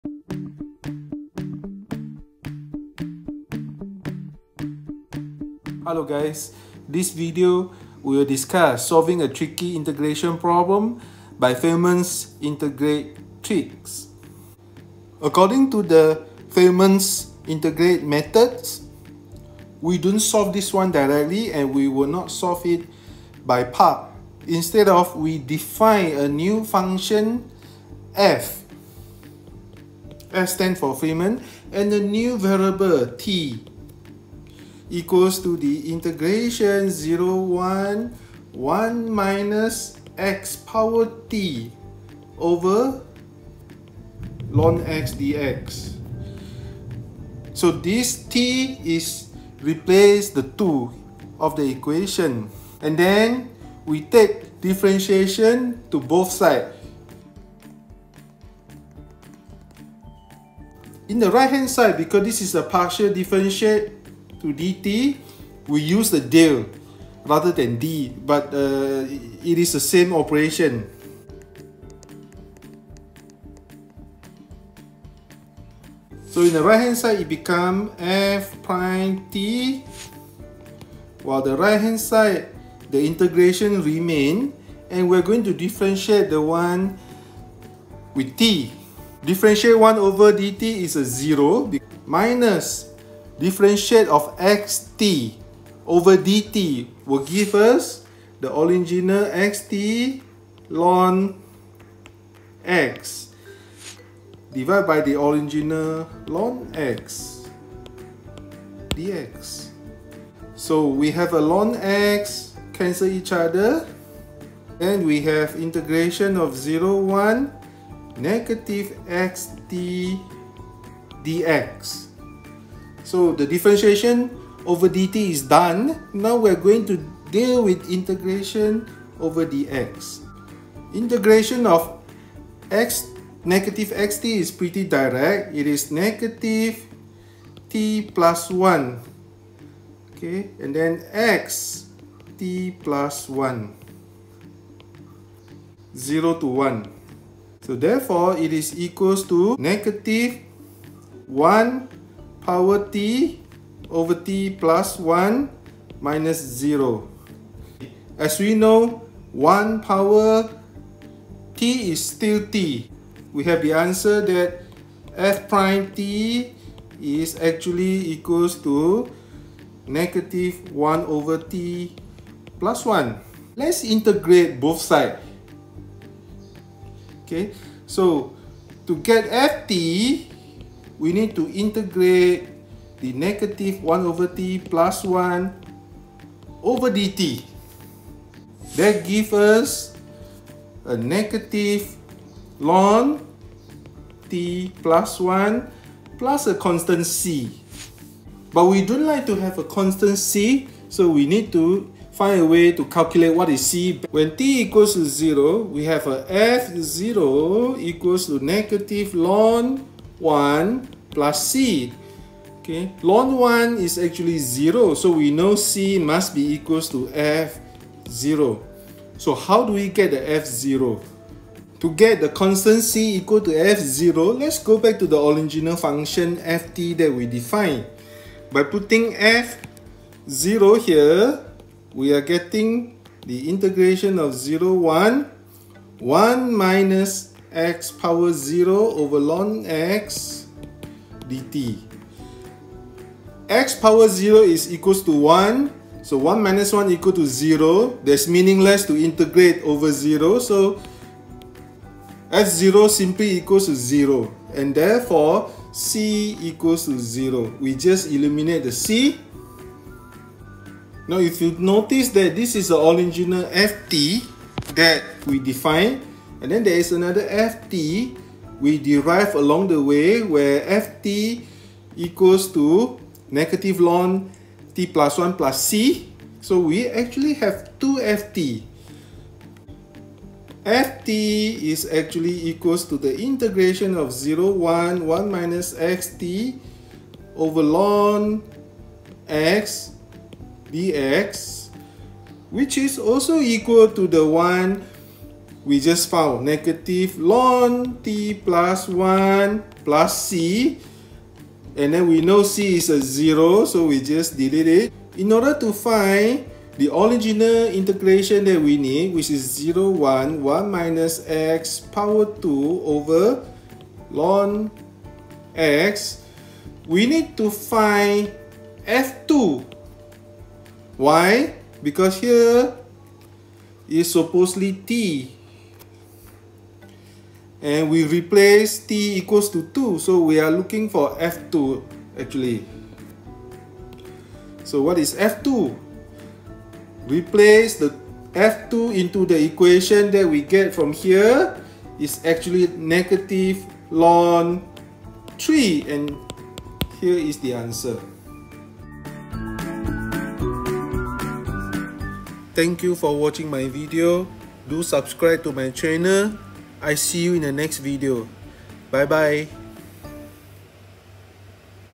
Feynman’s integral tricks for solving challenging integration problem.
This video demonstrate how to use Feynman’s integral trick to solve Integration of (1 - x^2)/ln(x) dx for x=0 to 1.